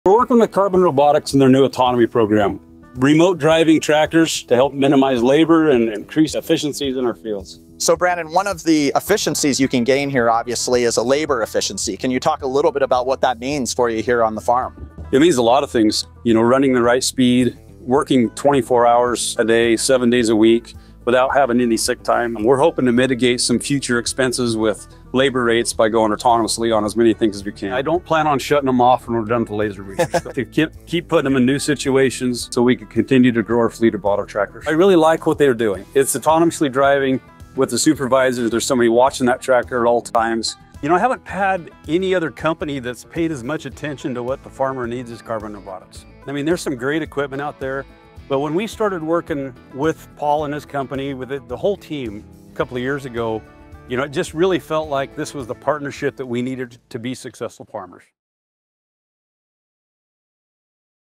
Carbon Robotics AutoTractor, with Brandon Munn
Carbon AutoTractor: Tractor Autonomy With Real-Time Remote Supervision. GeekWire Studios heads to Eastern Oregon for an exclusive look at Carbon Robotics’ AutoTractor, an AI-powered, autonomous tractor system revolutionizing agriculture. With 24/7 operation and remote monitoring, AutoTractor is changing the game for modern farming.

Discover how farmers across the U.S., Canada, Europe, and Australia are leveraging Carbon Robotics' technology to drive farm profitability and sustainability. Watch now!